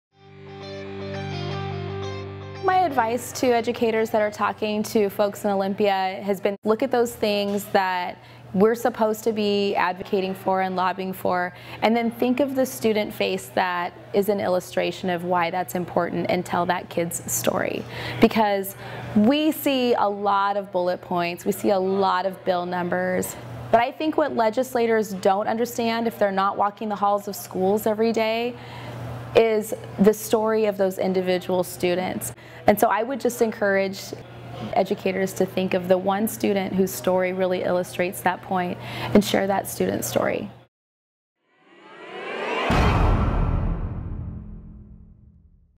Lobbying Tips for Washington Educators
WEA member Rep. Monica Stonier offers tips on how to lobby your legislators -- talk about your students' needs.